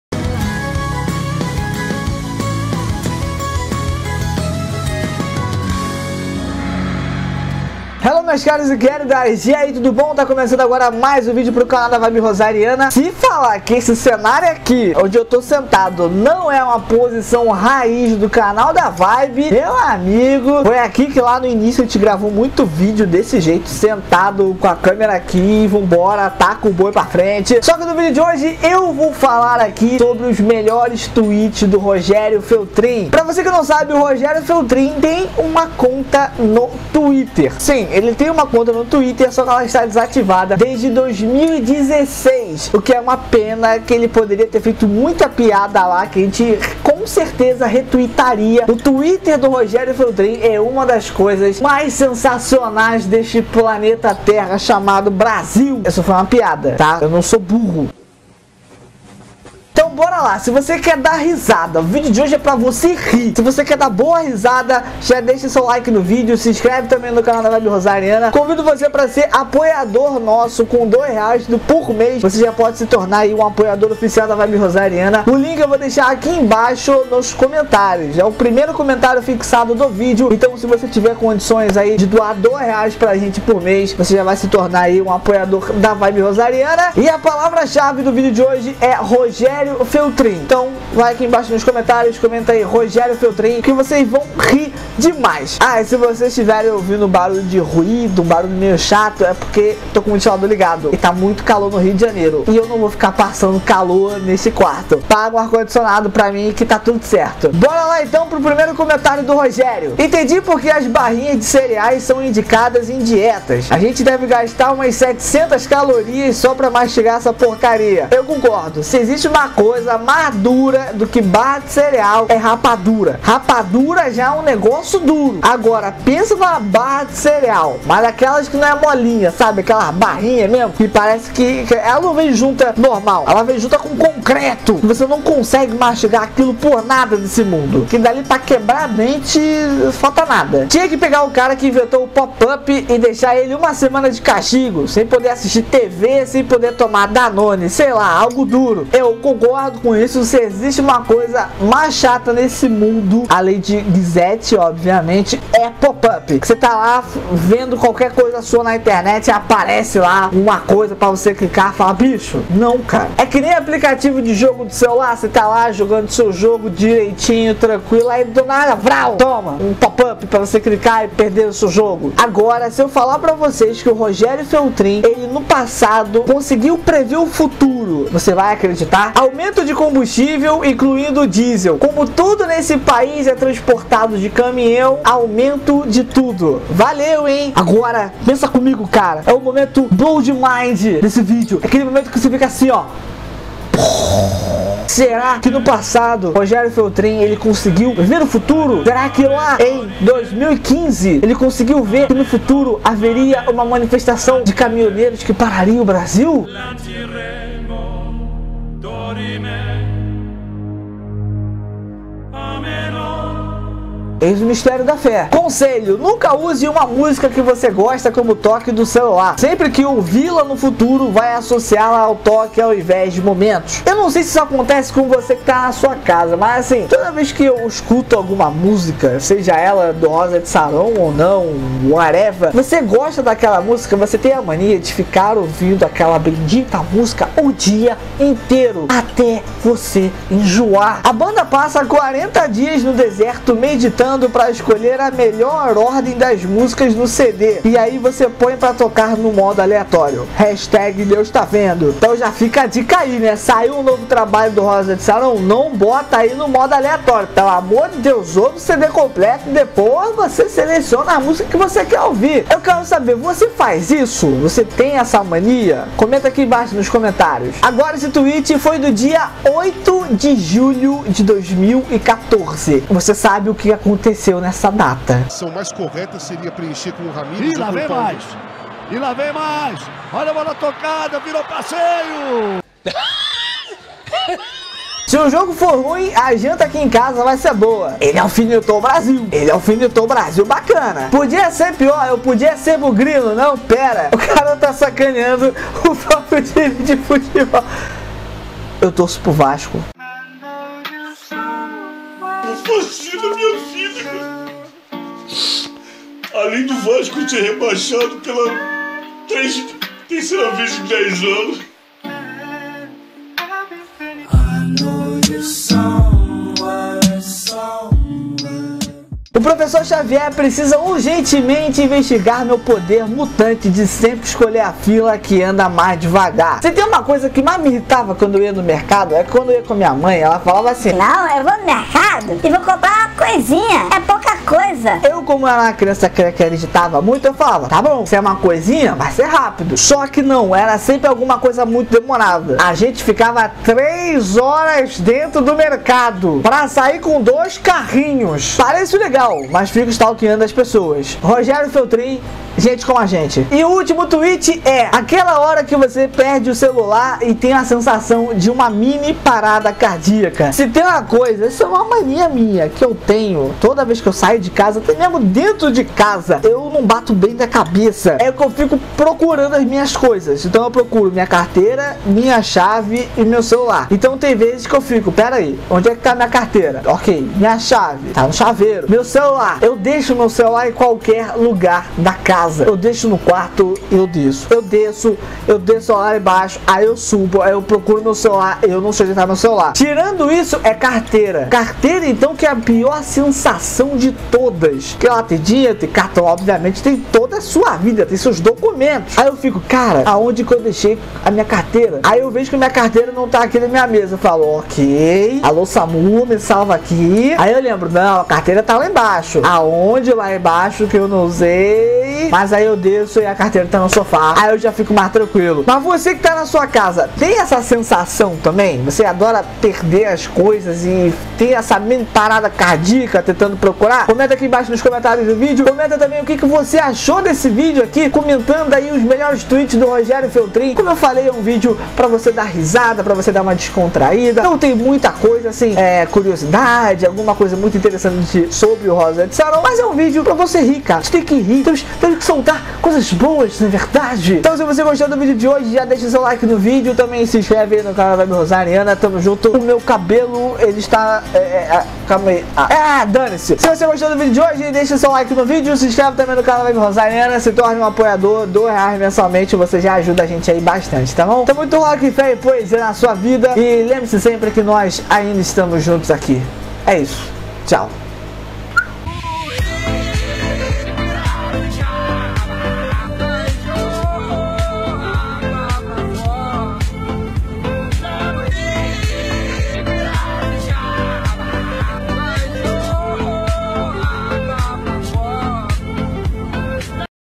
LENDO OS TWEETS MAIS ENGRAÇADOS DO ROGÉRIO FELTRIN
Os melhores e os mais engraçados tweets do Rogério Feltrin no Twitter.